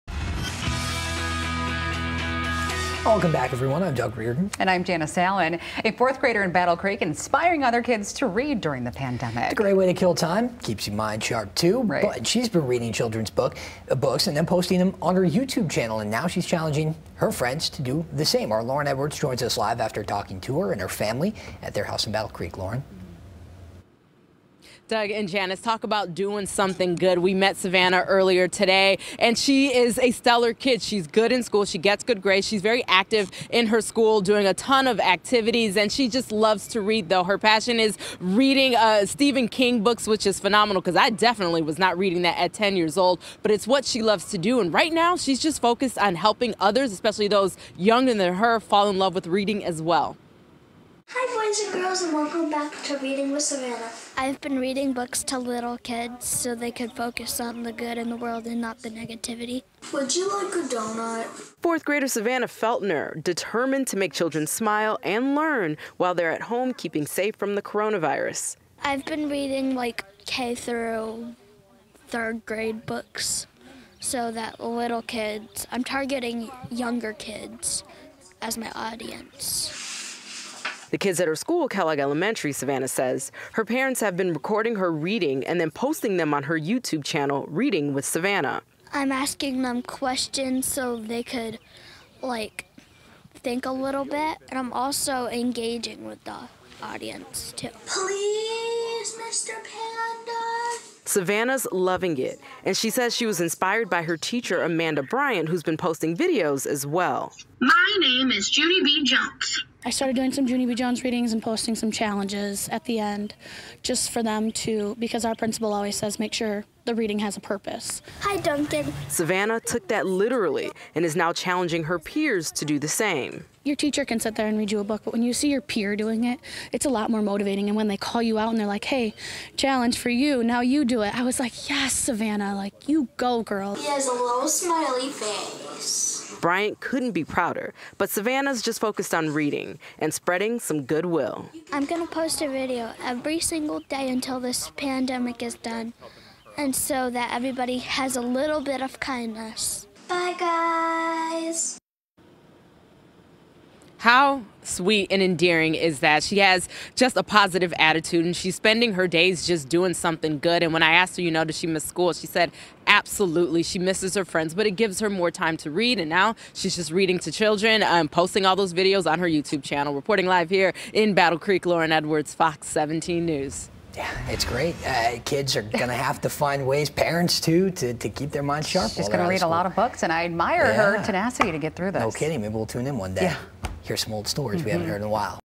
4th grader reading to kids ‘so they can focus on the good in the world’ amid pandemic
BCPS fourth grader Savannah Feltner is making YouTube videos of herself reading children's books in hopes of inspiring kids to fall in love with reading.